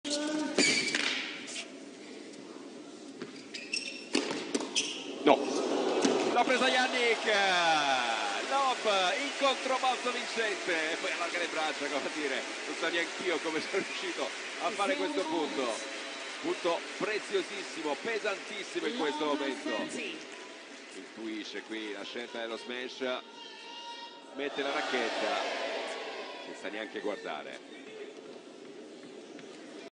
Incredibile giocata di Sinner, il colpo dell'anno!
Lob no look in controbalzo vincente, il giocatore di San Candido si e' reso protagonista di un incredibile giocata.  nell'incontro con Gombos.